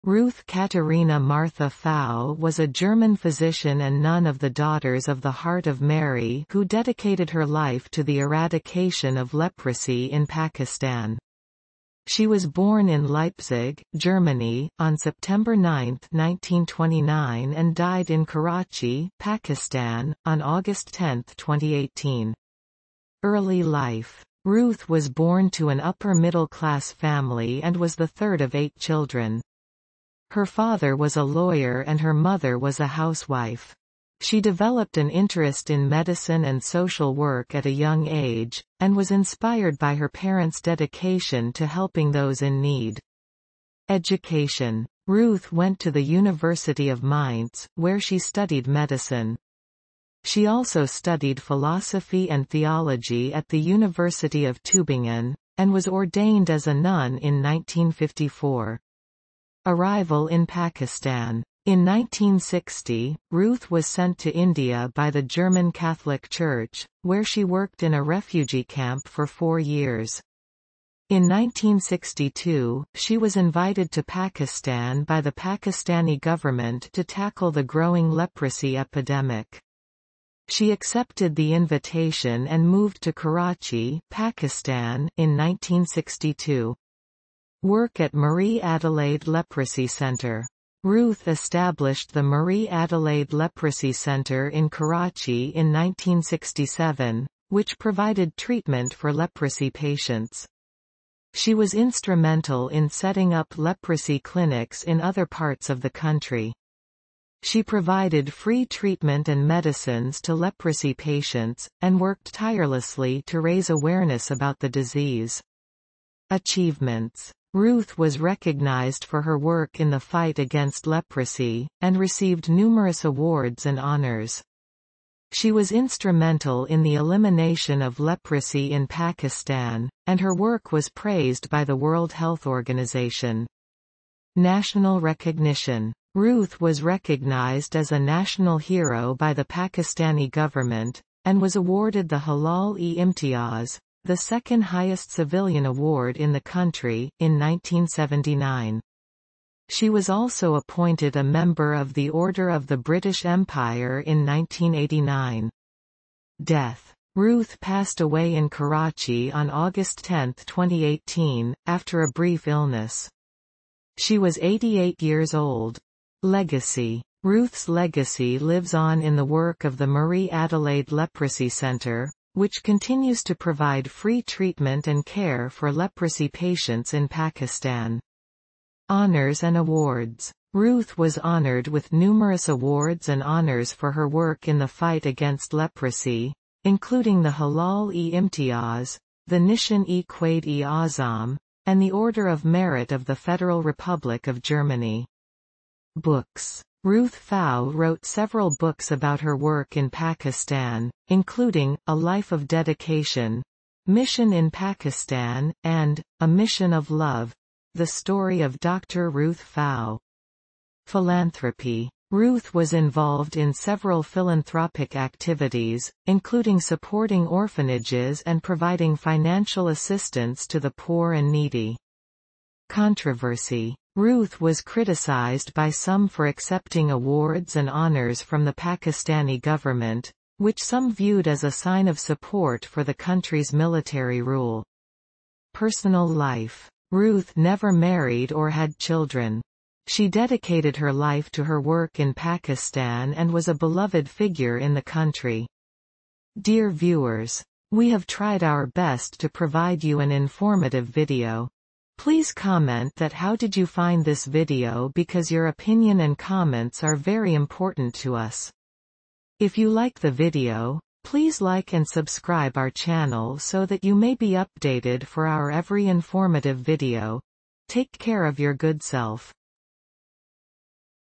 "Dr. Ruth Pfau: A Heroine Who Dedicated Her Life To Eradicating Leprosy in Pakistan"
The late Dr. Ruth Pfau, also known as the "Mother Teresa of Pakistan," devoted her life to providing medical care for those in need, free of charge. She was awarded Pakistan's highest civilian award, the Hilal-i-Imtiaz.